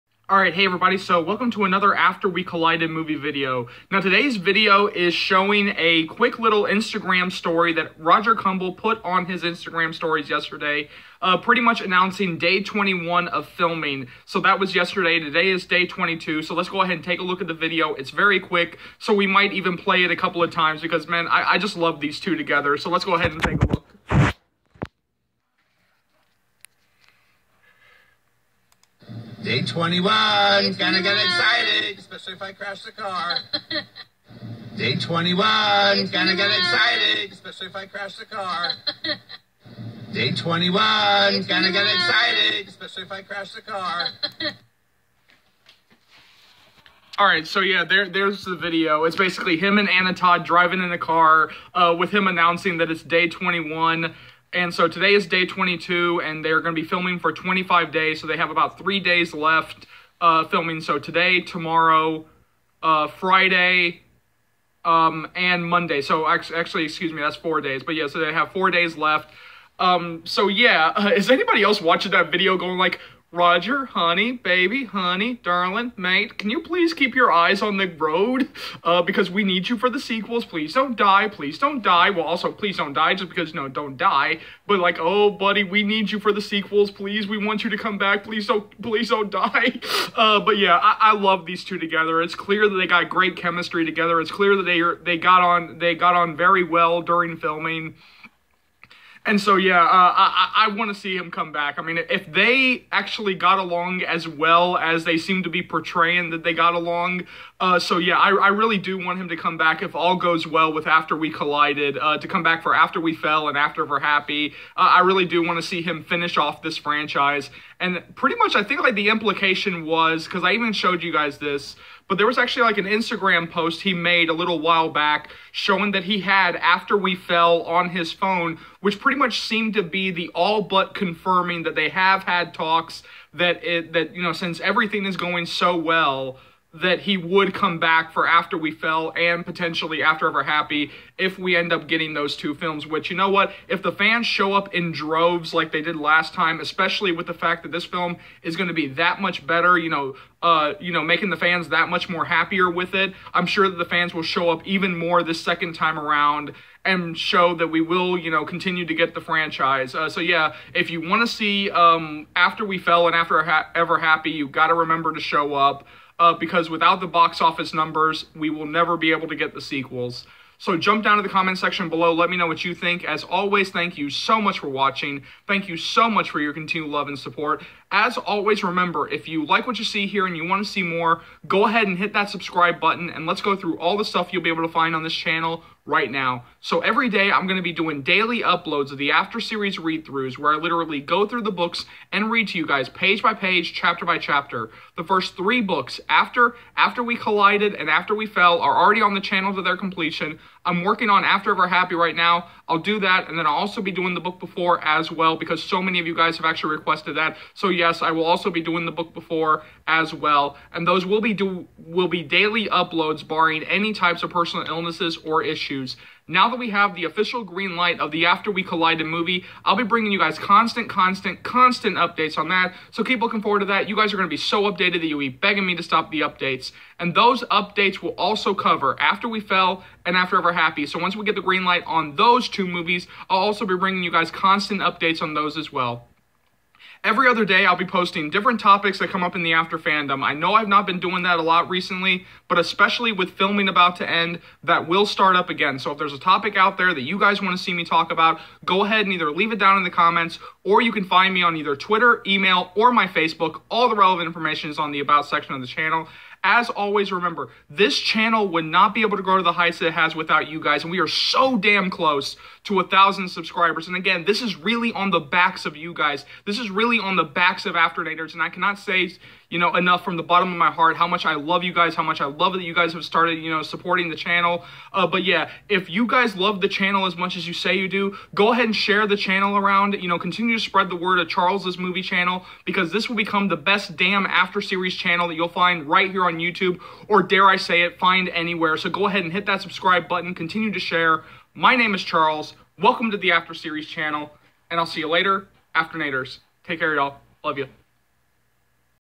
After We Collided Movie Roger Kumble Day 21 Instagram Video
Roger Kumble posts a quick Instagram Video on Day 21 of filming